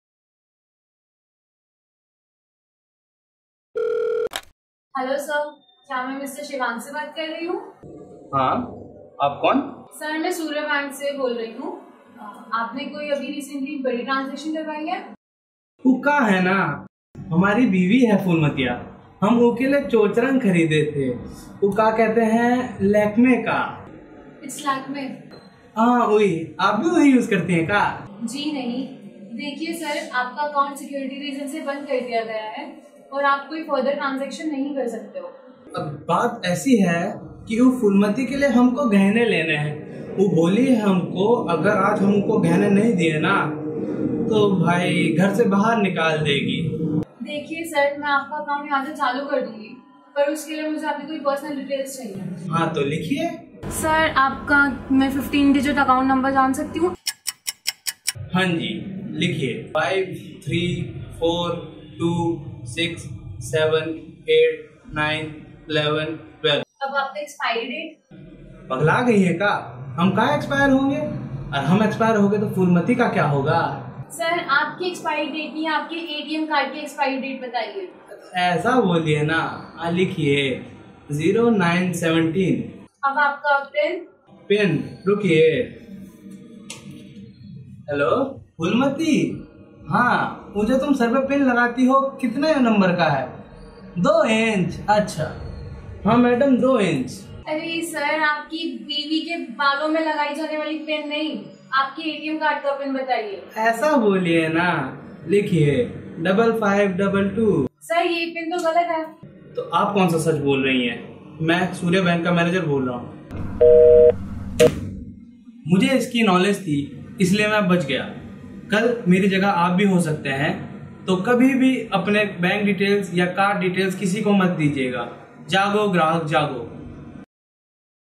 Jago Grahak Jago - Fake Bank Calls
Motive- create awareness among people to no to trust on these kind of fake calls.As they are fraud. Because person from bank do not ever call you to get your bank details.They talk in a way that any person starts believing on them and then they easily cheat you.
So be aware Be awake!